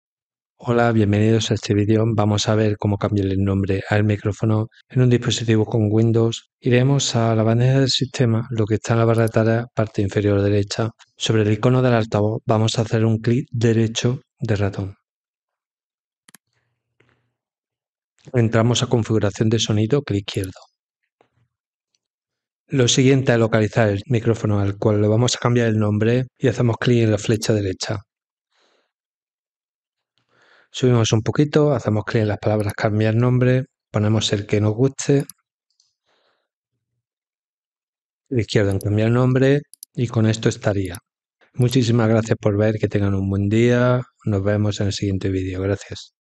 Como renombrar el nombre de tu micrófono en tu PC
Videotutorial fácil y rápido de como ponerle el nombre que tu quieras a tu micró en los ordenadores con Microsoft Windows 11 y 10.  Si tienes varios micrófonos y cambias habitualmente de dispositivo de entrada de audio este tutorial te viene como anillo al dedo ya que en ese caso es mejor tener cada micro con un nombre con el que estemos más familiarizados.